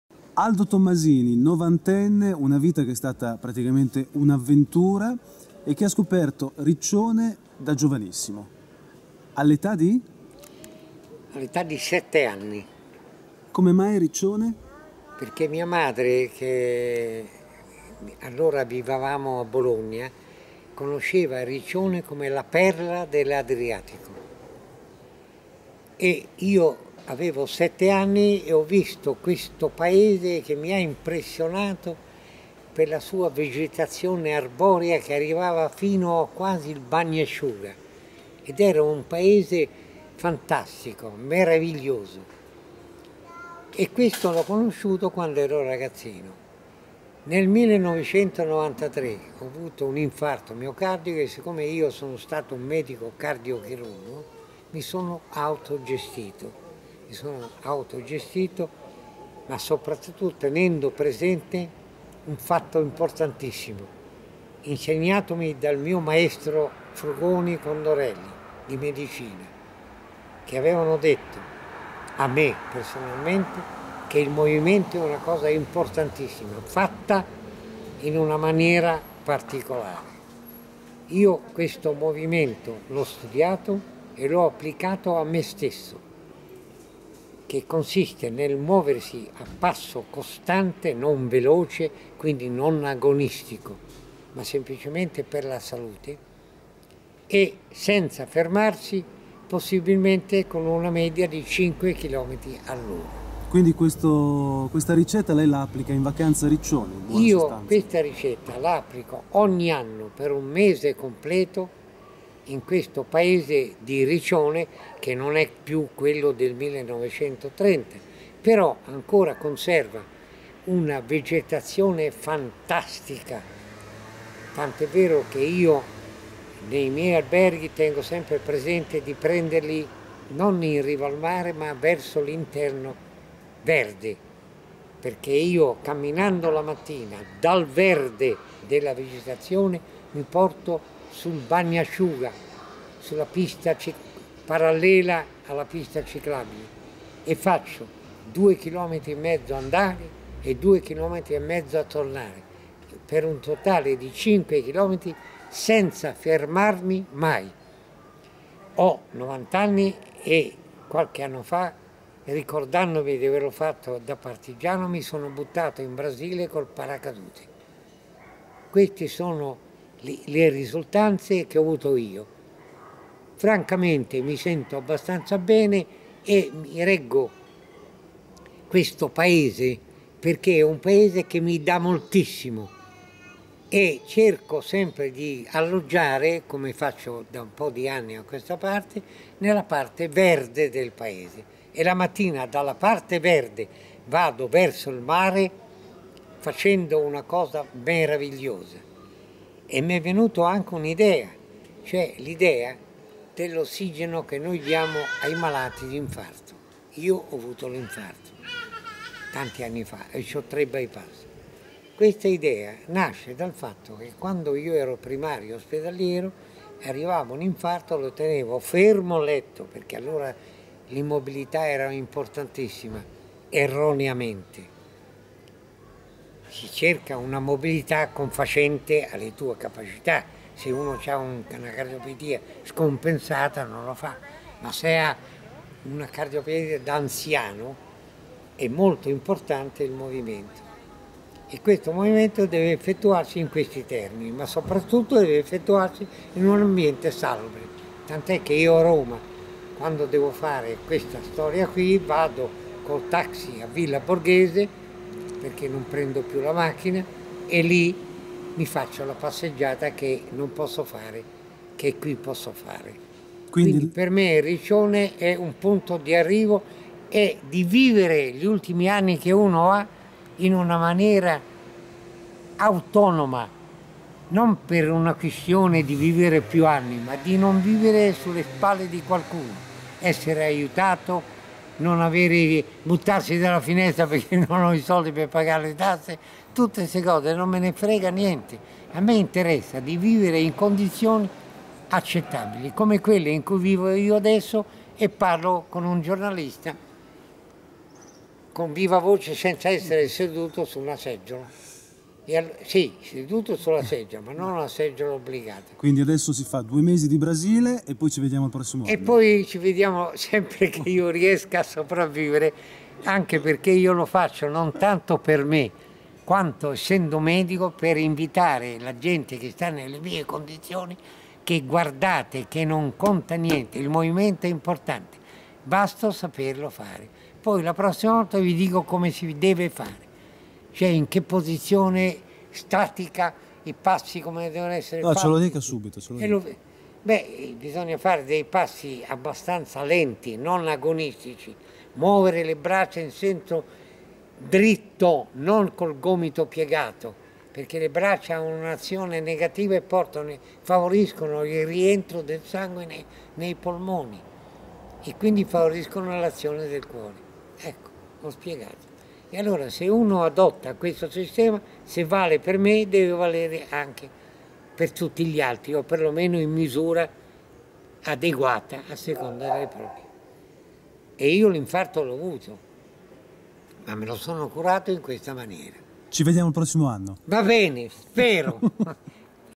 Aldo Tommasini: un 90 enne come pochi. Riccione la sua palestra a cielo aperto !
Aveva 7 anni quando per la prima volta venne in vacanza a Riccione. Aldo Tommasini romano 90 enne pieno di energia e protagonista di una vita densa di esperienze, quasi un romanzo. "Avevo vent'anni quando mi mandarono a tagliare i telefoni al comando tedesco. Era la mia prima azione partigiana. La paura, che era tanta, la superai anche per non fare brutta figura con la bella ragazza che mi accompagnava". Aldo allora era agli ordini del nonno di Luca di Montezemolo nella Resistenza romana. Un uomo instancabile e dalle mille risorse con due occhi ancora pieni di vitalità. Primario, docente universitario, cardiologo, e prima ancora paracadudista, partigiano e... cantante (il suo sostituto era Marcello Mastroianni). Un testimone di un pezzo di storia italiana, quella vera, vissuta in prima persona. "Giuro, ho sentito su Radio Londra Togliatti ed il Re trovare un accordo sul come uscire dalla guerra per il bene del nostro paese martoriato e allo sbando". Per Aldo, madre medaglia d'oro della Resistenza, un fratello figura importante nel movimento anarchico degli anni Venti ed un altro, tenente dei carabinieri, fucilato alle Fosse Ardeatine la vita è stata zeppa di emozioni. Anche nella professione di primario di cardiologia accompagnata poi dal tardo amore per l'astronomia ed una perenne grande sete di sapere. Tutto questo, e tante altri aneddoti per due ore abbondanti di chiacchierata con Aldo Tommasini anche per questa Estate a Riccione sua autentica palestra a cielo aperto.
Che sia il segreto per arivare così in forma a 90 anni? Aldo non ha dubbi l'attività fisica, anche per chi ha tre by pass è fondamentale !. E se lodice lui. c'è da credergli !